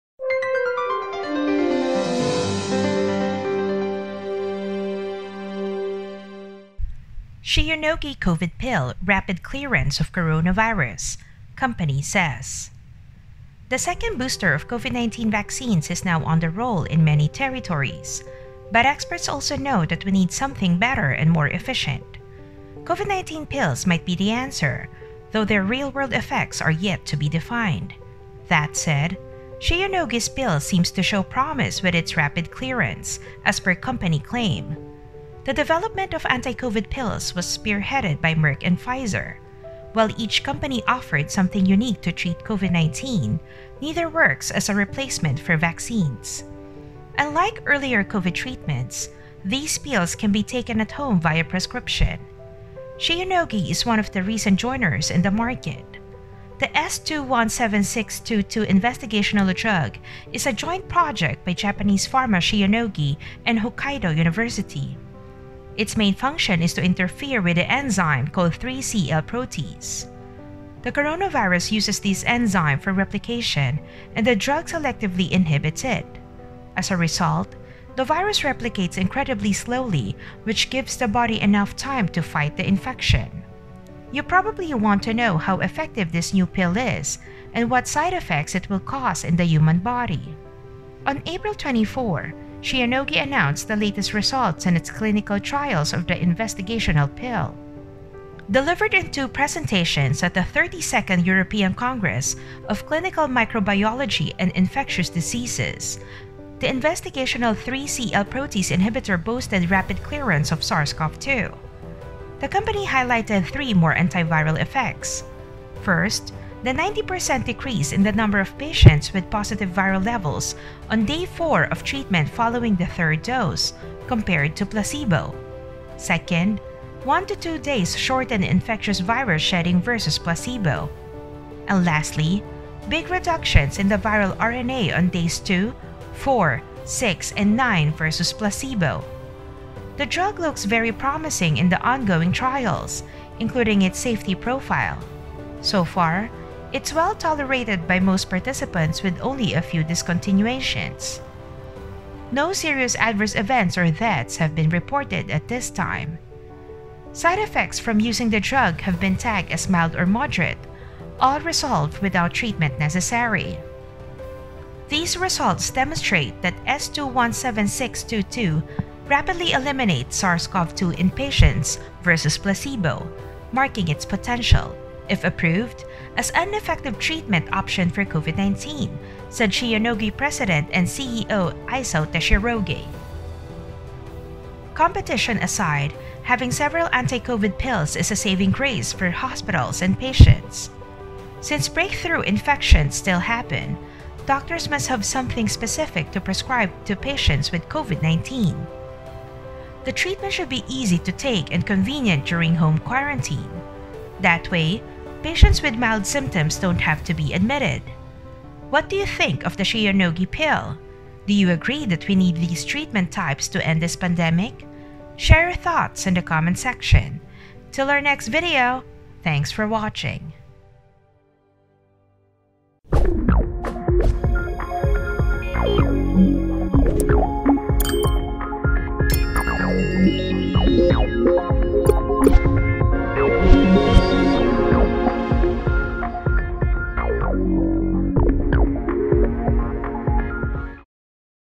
Shionogi COVID Pill "Rapid Clearance" of Coronavirus, Company Says
shionogi covid pill, covid-19 pill, covid pill shionogi, covid treatment, covid medicine

Shionogi's investigational anti-COVID pill shows great potential in a recent presentation.